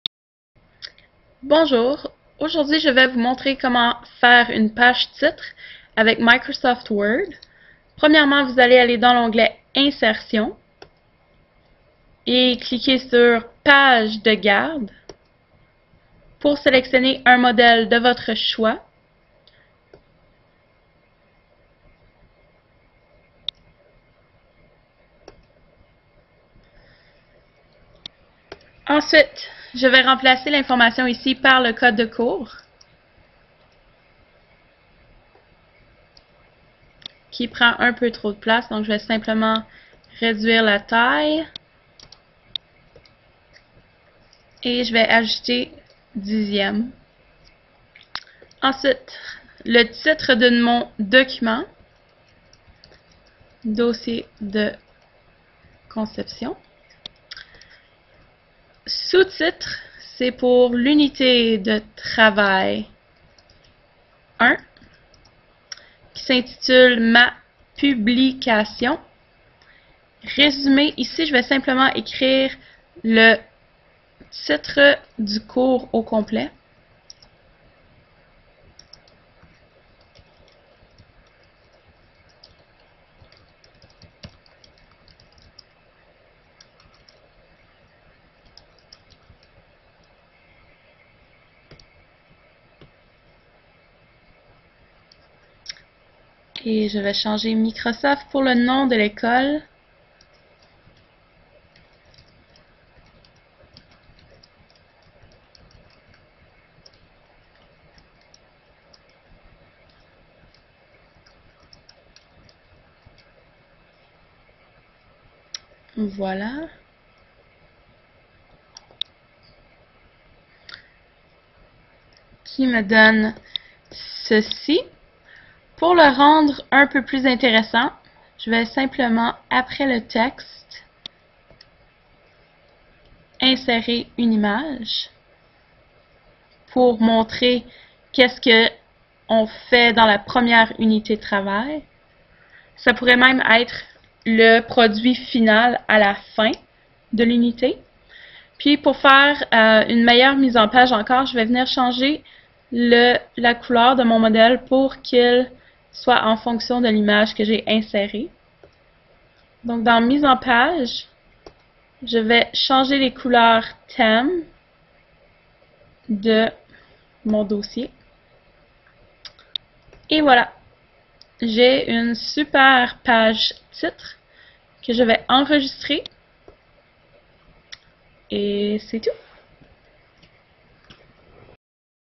La page titre du Dossier de Conception
Comment faire un page titre avec le logiciel Microsoft Word